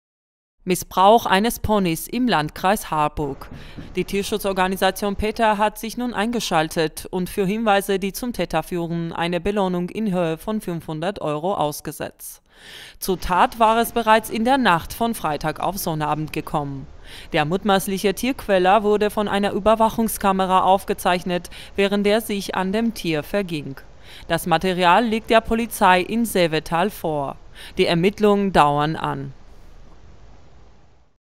Nach Missbrauch eines Ponys: Polizei sucht Zeugen
Missbrauch eines Ponys im Landkreis Harburg. Die Tierschutzorganisation PETA hat sich nun eingeschaltet und für Hinweise, die zum Täter führen, eine Belohnung in Höhe von 500€ ausgesetzt.
Zur Tat war es bereits in der Nacht von Freitag auf Sonnabend gekommen. Der mutmaßliche Tierquäler wurde von einer Überwachungskamera aufgezeichnet, während er sich an dem Tier verging. Das Material liegt der Polizei in Seevetal vor. Die Ermittlungen dauern an.